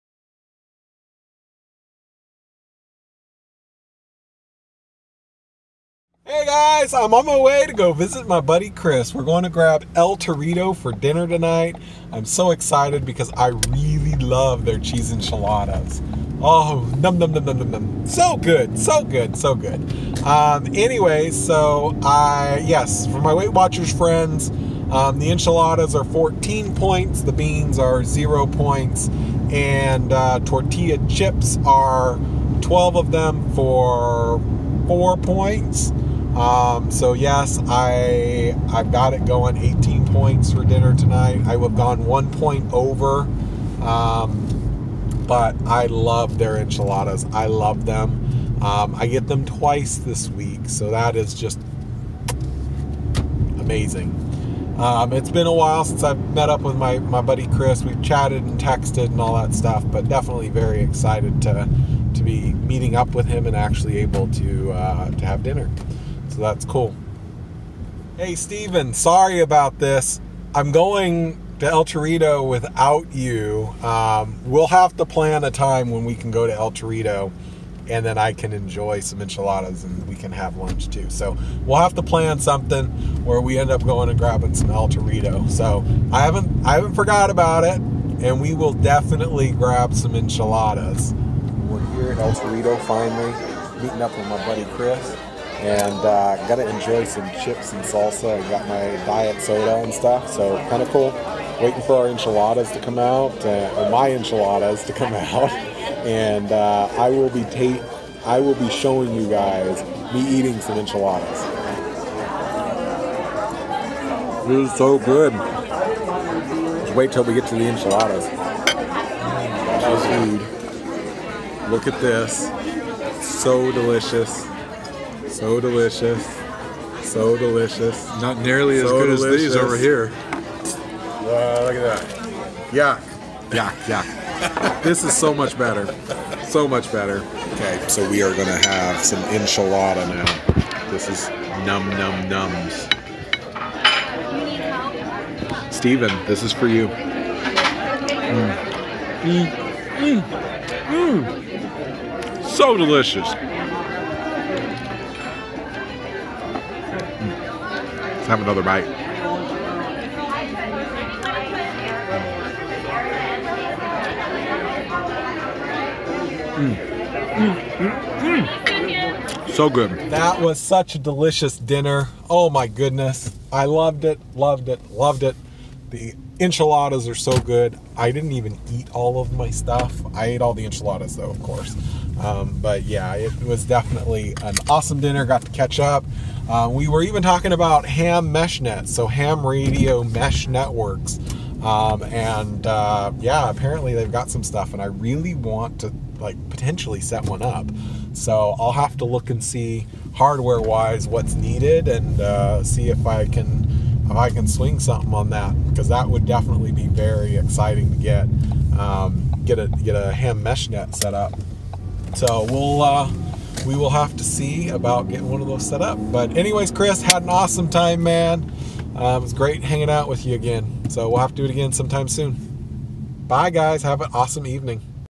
El Torito Dinner Vlog April 11, 2018
Enjoyed an awesome Dinner at El Torito with my buddy Chris. Checkout the awesome Cheese Enchiladas I enjoyed tonight.